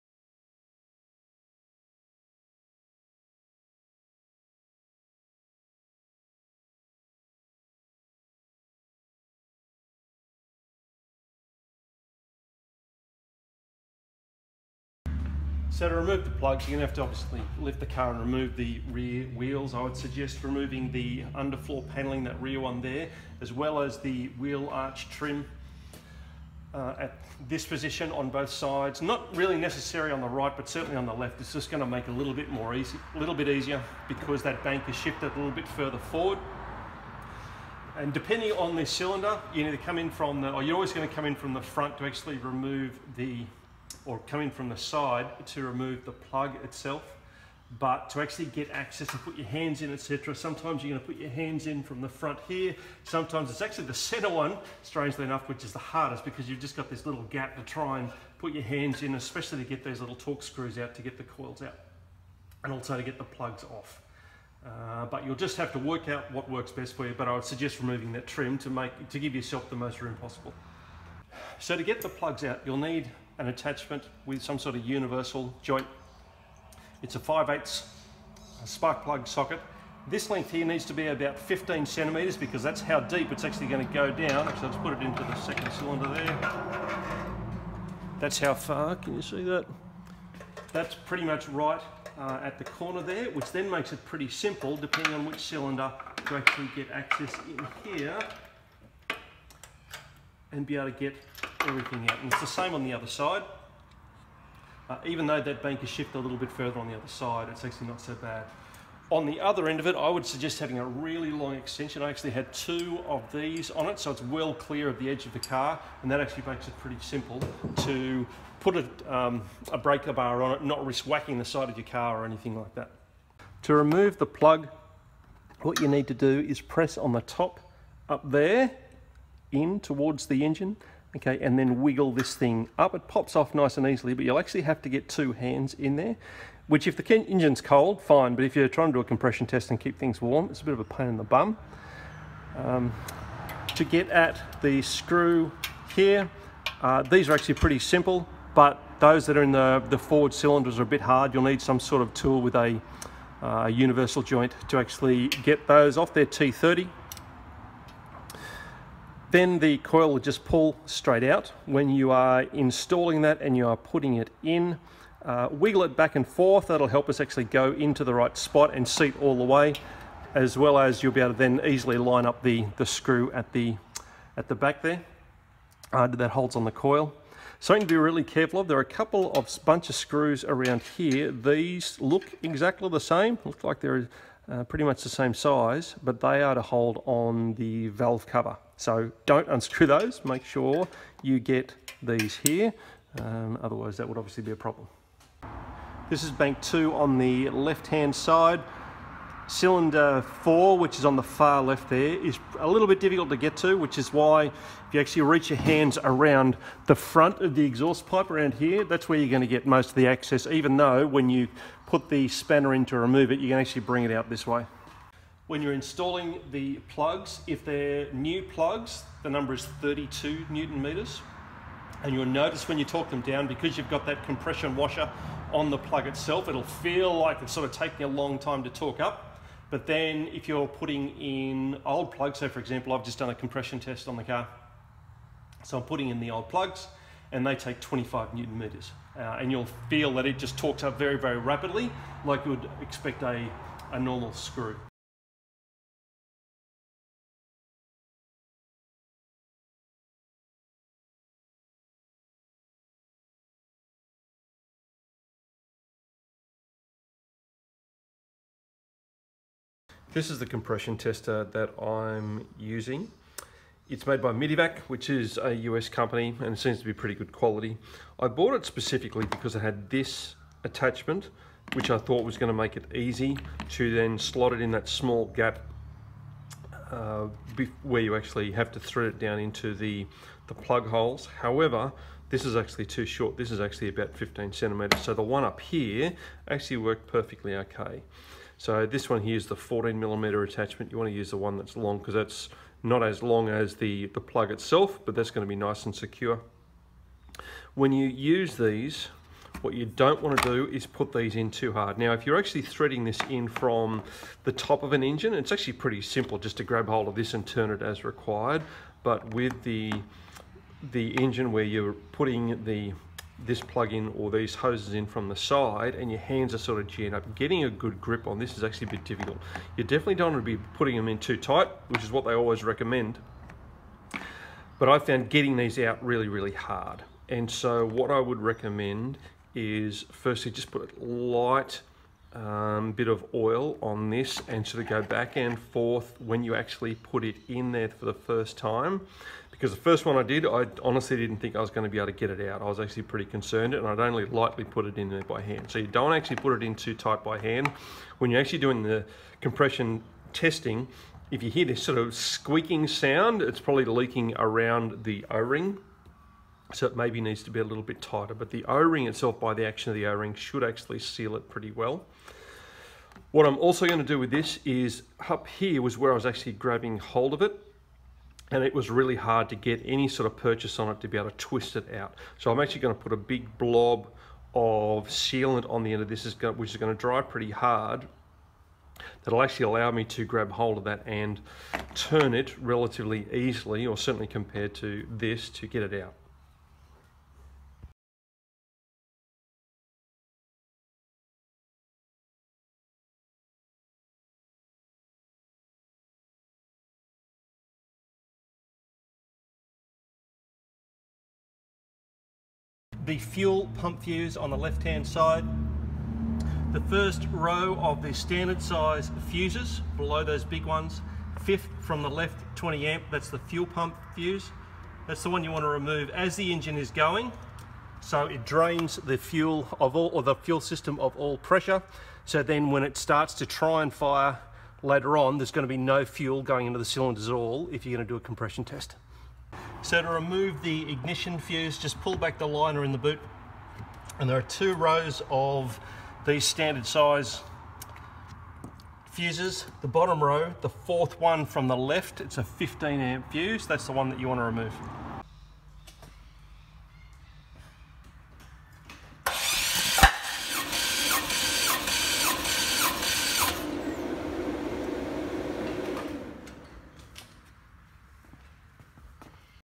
981 Spark plugs, compression test
How to replace the spark plugs and also do a compression test on a Porsche 981 Boxster